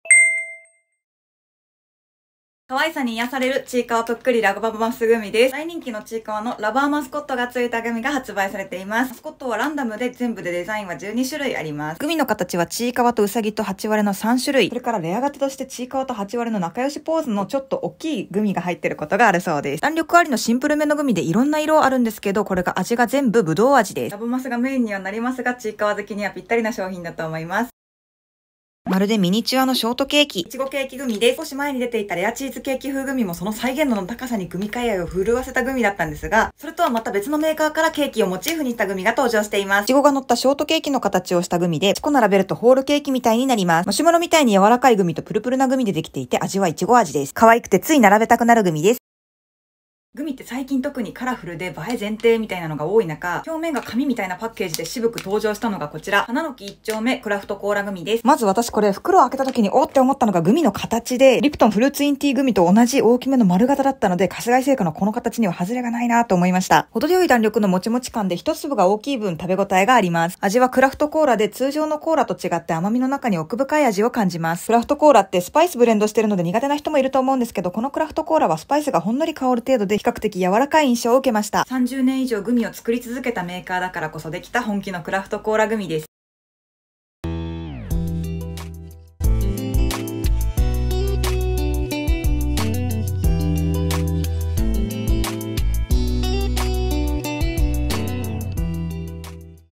【短くグミ紹介】ちいかわグミ／いちごケーキグミ／花の木一丁目クラフトコーラグミ
9月12日-9月17日
TikTokのグミ紹介動画まとめです

【購入店舗・金額】

1.ちいかわぷっくりラバマスグミ(キラキラドンキ¥330)

2.いちごケーキグミ(おかしのまちおか¥85)

3.花の木一丁目クラフトコーラグミ(LOFT¥？？？)

()内に購入店舗と購入金額記載
※わたしが買ったものを書いてるので
  ほかにも取り扱い店舗・金額の異なる店あります

【あいうえお🌈🍇💖グミ】
日本グミ協会会長
Twitter、Instagram、TikTokでもグミ紹介してます